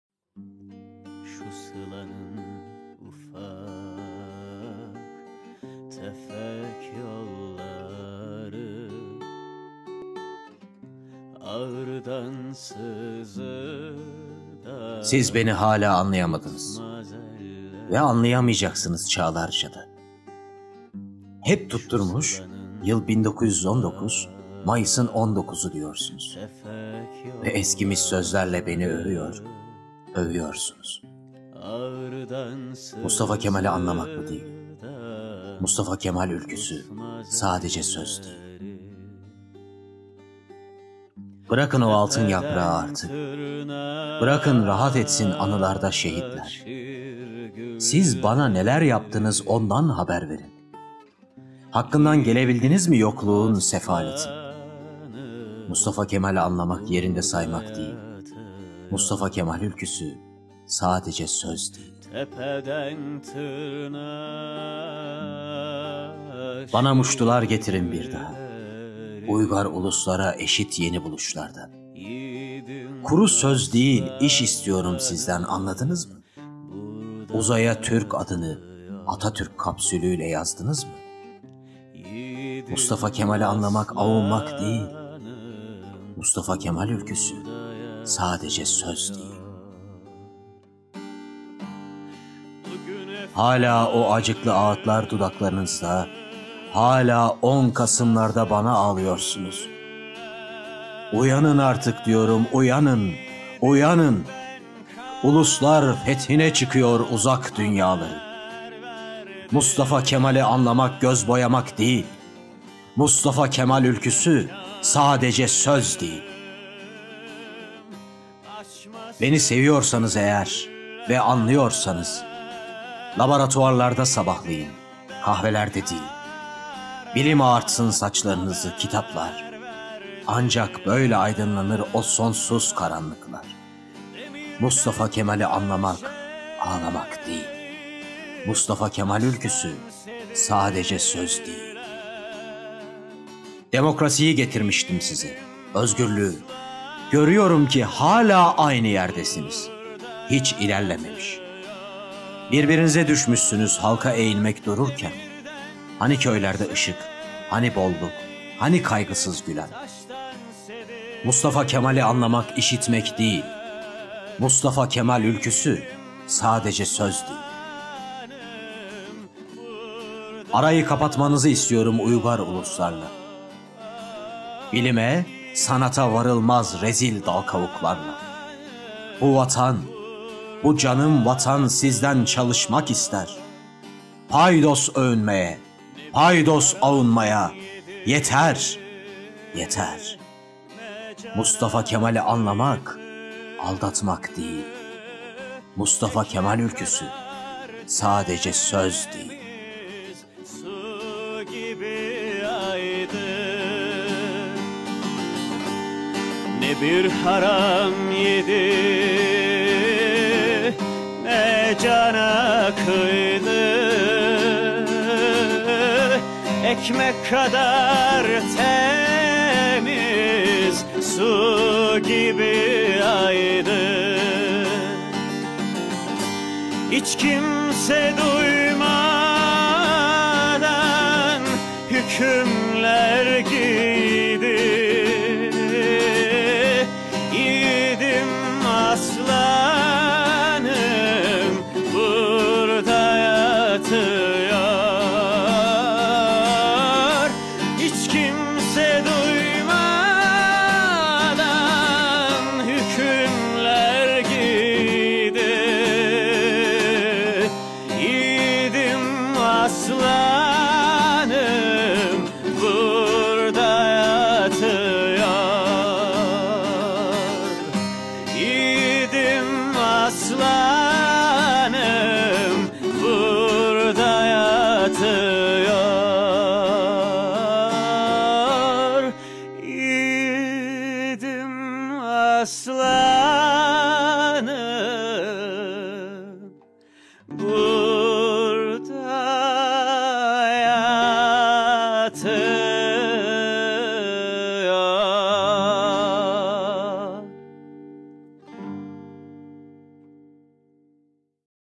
Atatürk'ten son mektup - Halim Yağcıoğlu | Ahmet Bozdağ
Benim naçiz vücudum elbet bir gün toprak olacaktır fakat Türkiye Cumhuriyeti ilelebet payidar kalacaktır. Ulu Önder Gazi Mustafa Kemal Atatürk'ü saygı, sevgi ve özlemle anıyoruz.

Seslendiren: Emre Şahin
Şiir: Atatürk'ten Son Mektup
Seslendiren: Ahmet Bozdağ